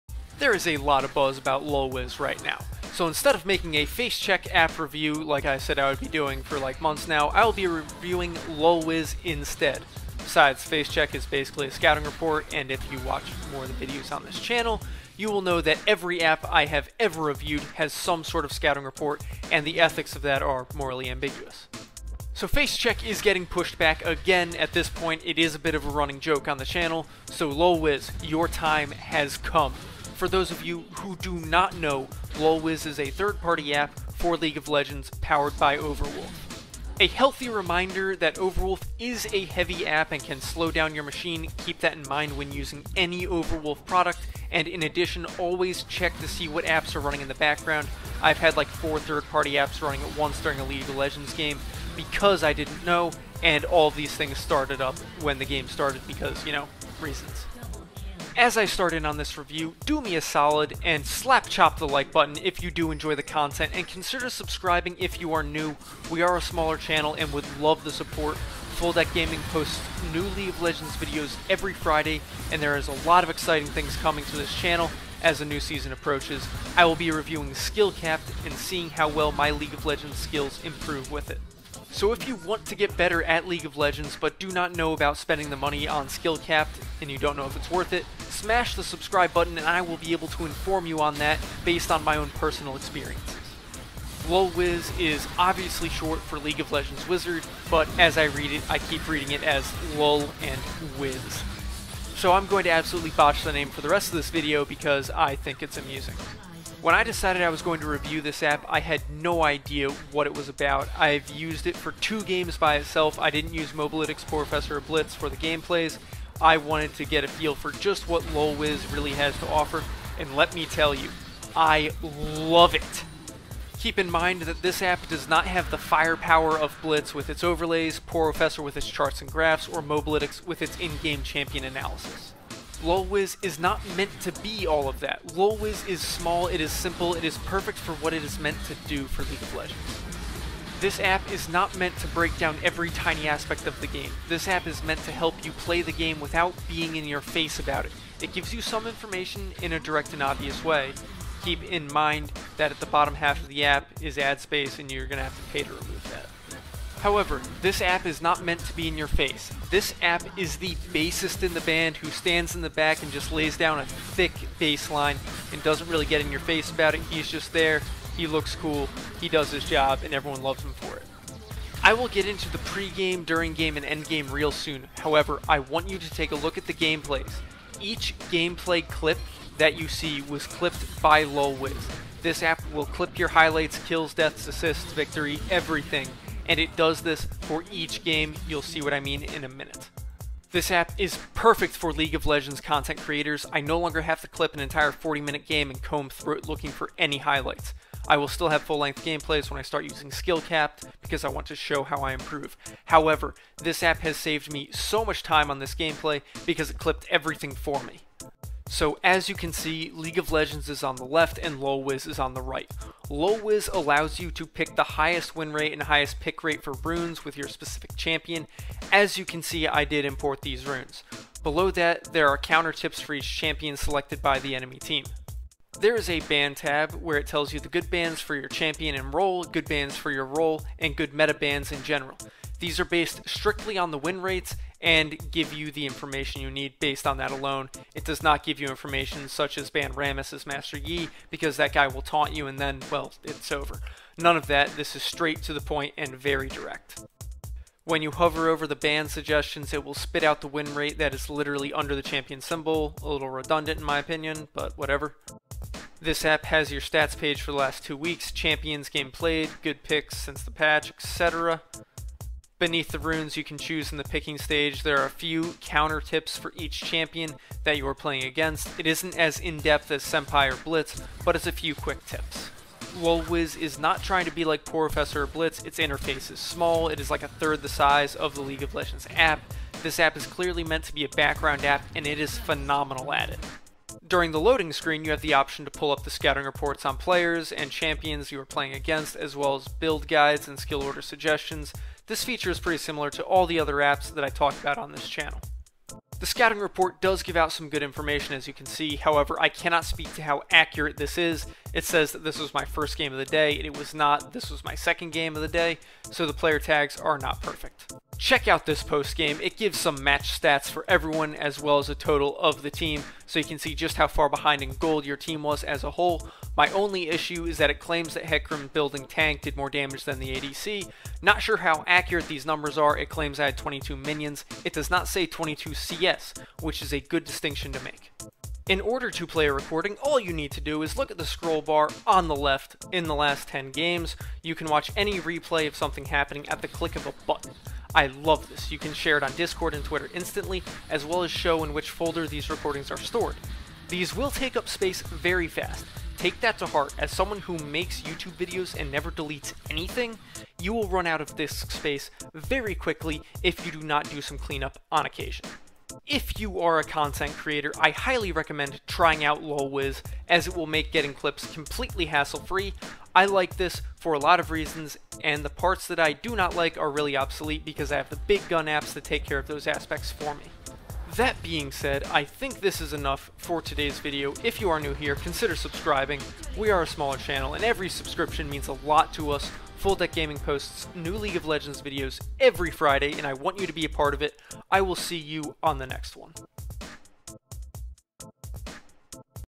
LolWiz League of Legends App Review [Third Party App]
LolWiz is a free to use app by Overwolf. This app is great for content creators and those who want to be able to make clips that seem to be much more game focused than analysis focused. Lolwiz gives gamers the ability to play league of legends with a very much hands off feeling from the third party app. Lolwiz is excellent and easy to use.

0:00 Intro
1:30 LolWiz Overview
3:15 Pre Game
4:44 Scouting Report
5:08 Post Game
5:29 Recordings
6:11 Outtro